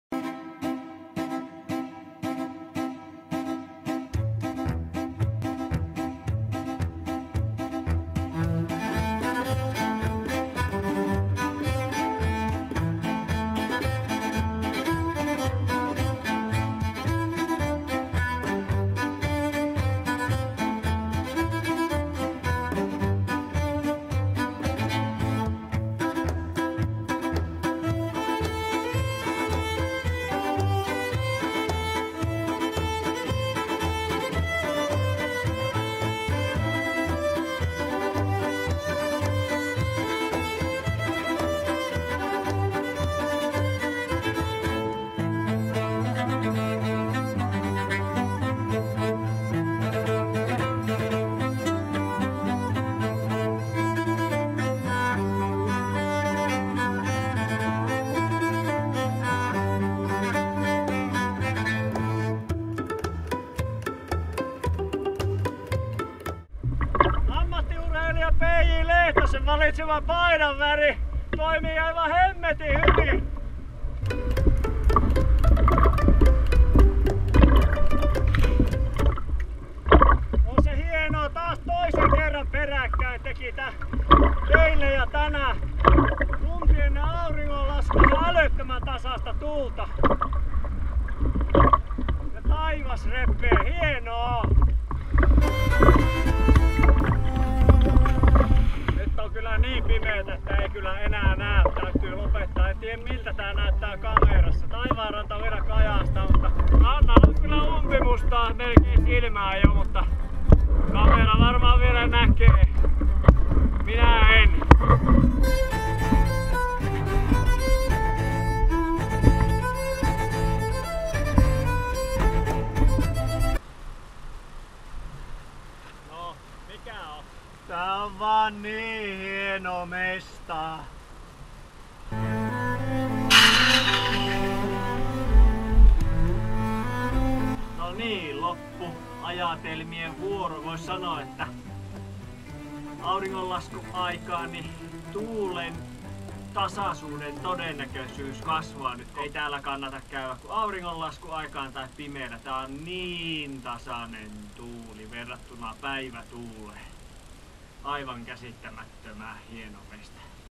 KitePerjantaiLauantaiPolkka 30 9 1 10 2016
Muuratjärvi tarjosi parastaan auringonlaskun aikaan ja sen jälkeen.